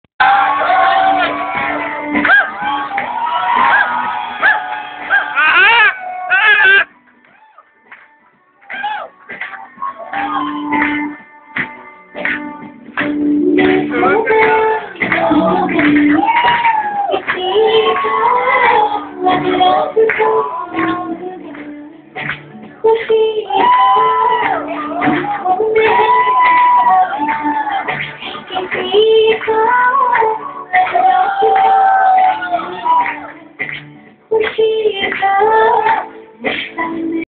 salil at his sangeet. now we can be assured...woh apni biwi ke unglion pe achha naachega!